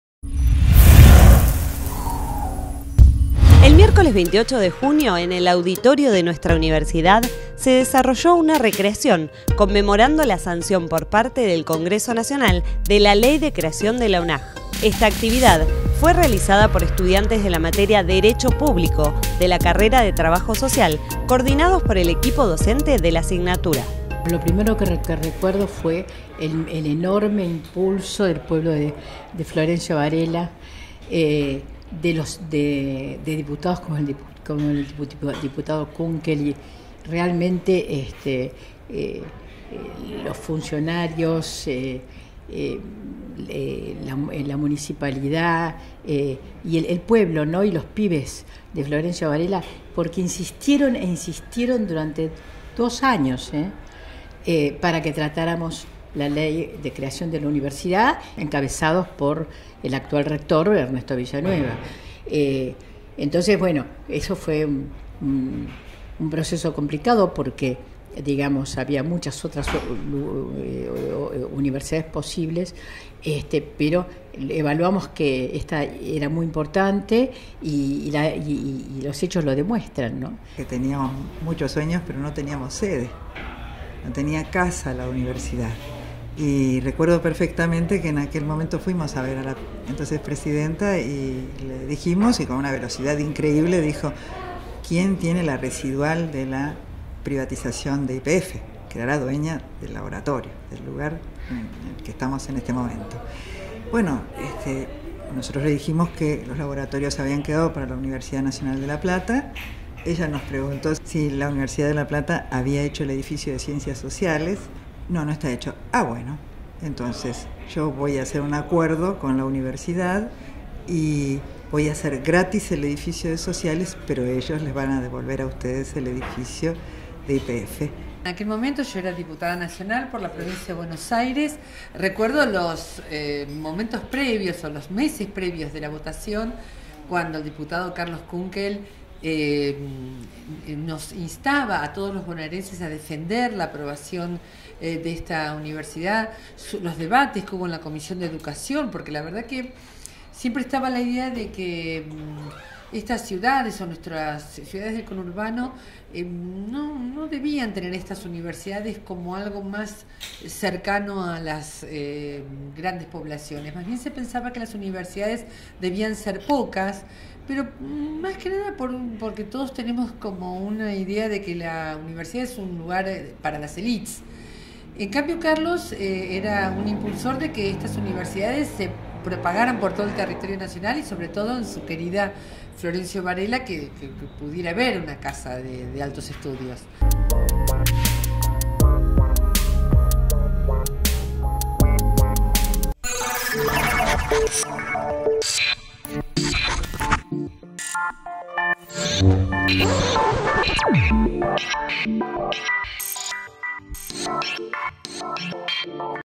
Estudiantes recrearon la aprobación de la ley de creación de la UNAJ
El miércoles 28 de junio en el auditorio de nuestra universidad, se desarrolló una recreación conmemorando la sanción por parte del Congreso Nacional de la Ley de creación de la UNAJ. Esta actividad fue realizada por estudiantes de la materia Derecho Público de la carrera de Trabajo Social, coordinados por el equipo docente de la asignatura.